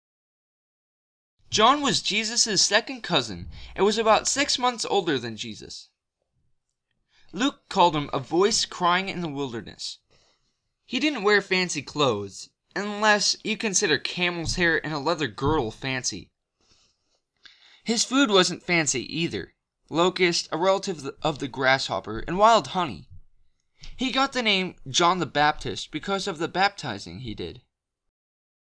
John the Baptist - Fact Sheet
We finally have a fact sheet for John the Baptist, though there are a few errors in it, some edges aren't being detected correctly, we're going to get those fixed, and maybe tweak some animation a bit, but this is near the final version that will be in the film.